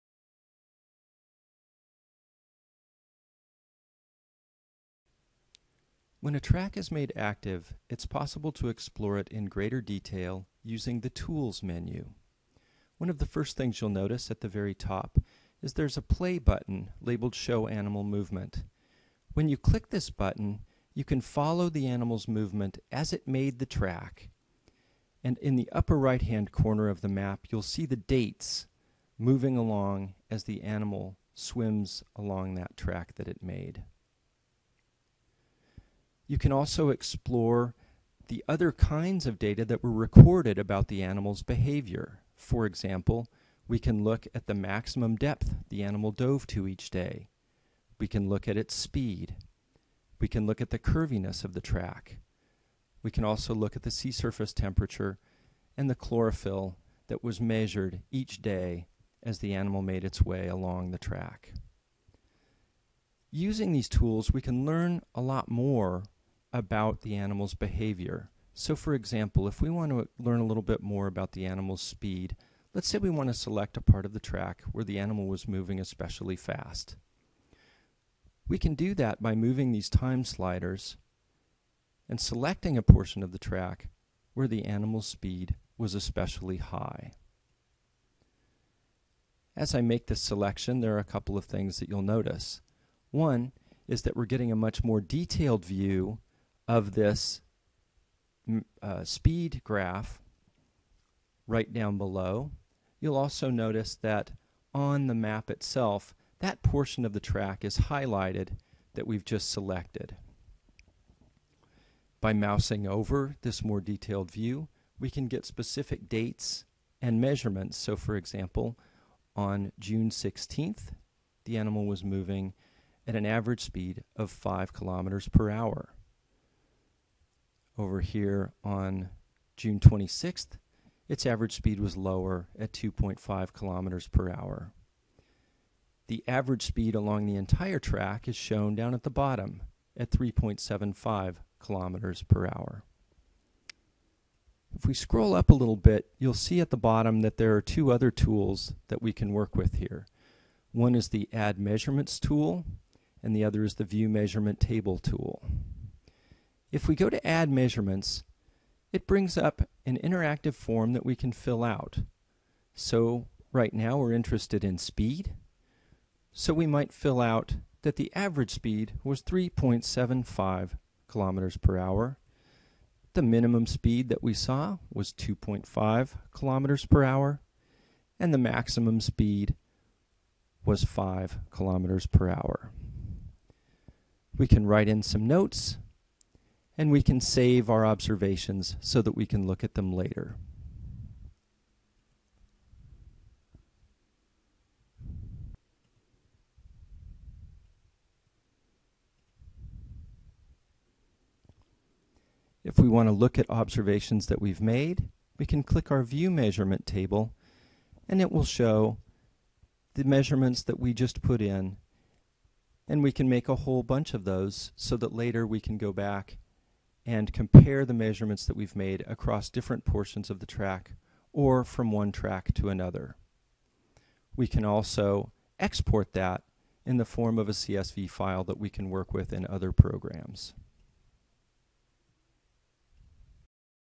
Ocean Tracks Instructional Video - The Tools
This video provides a brief overview of how to use the Tools menu in the Ocean Tracks interface.